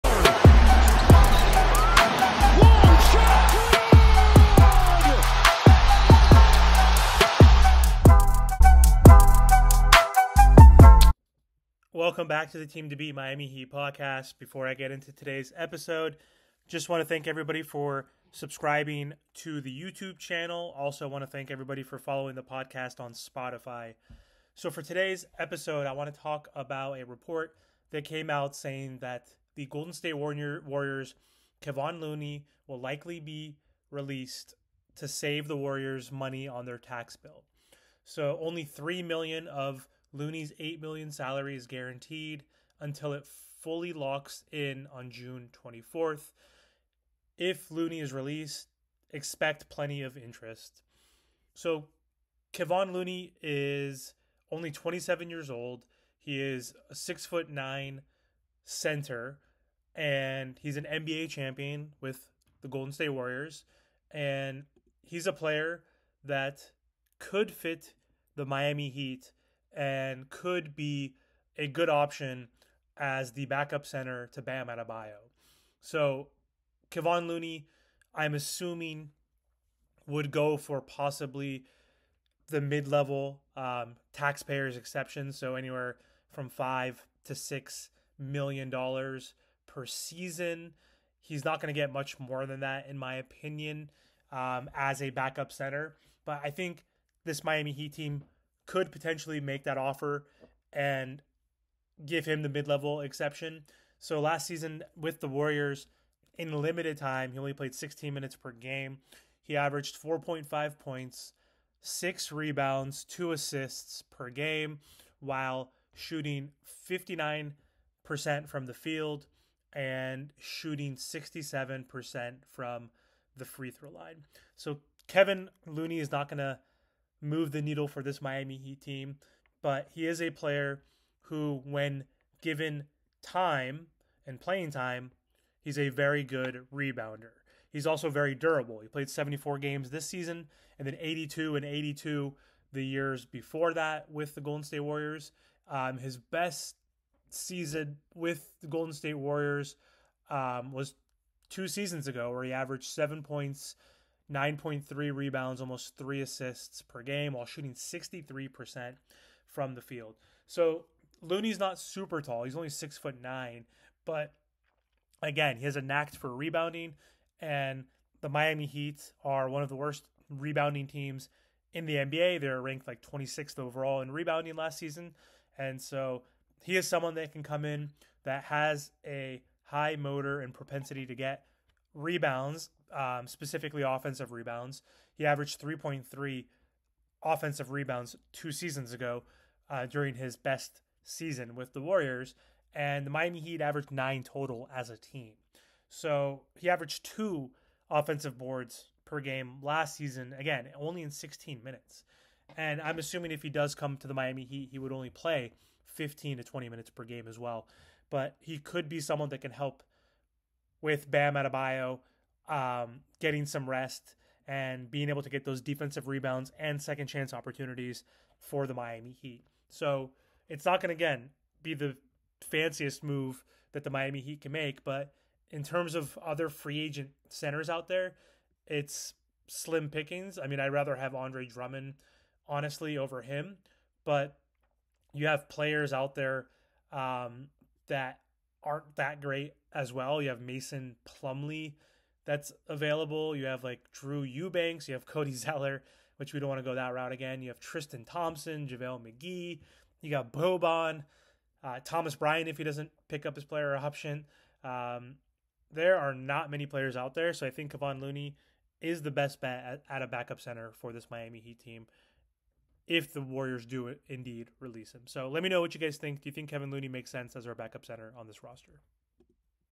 Miami Heat Rumors: The Miami Heat should sign Kevon Looney If the Warriors release him
A recent report suggested that the Golden State Warriors may release Center Kevon Looney. If so the Miami Heat should sign the former NBA champion.